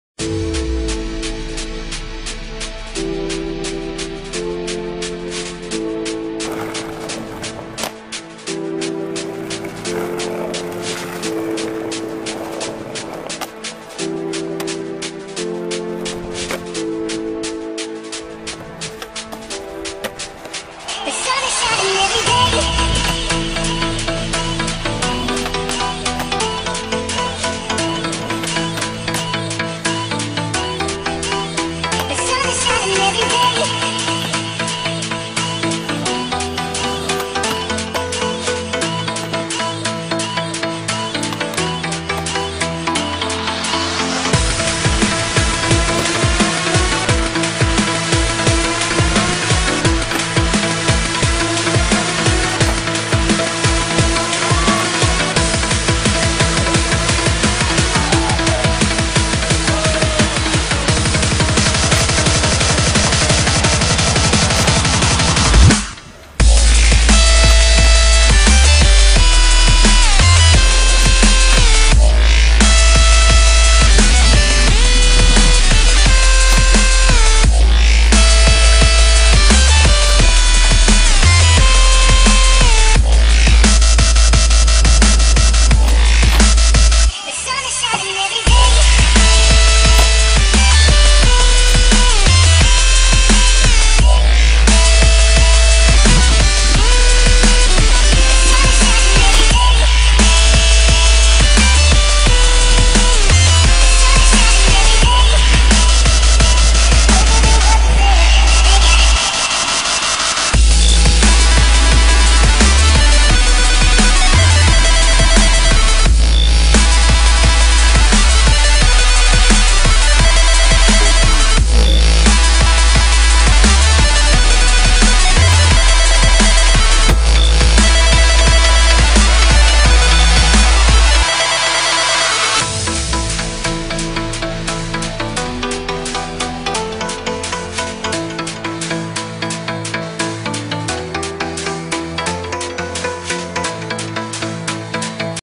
Chilling in hokitika with a skateboard.
first video made with very bad camera will use better one next time thanks.